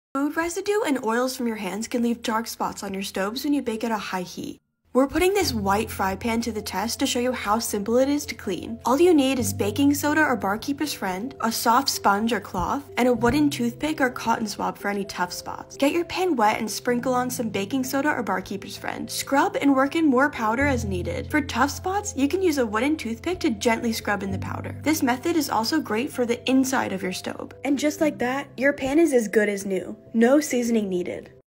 How to Clean your Staub Cast Iron Frypan
Stubborn stains on your Staub Frypan? We have a simple solution to easily clean it and  keep your masterpiece shining like new!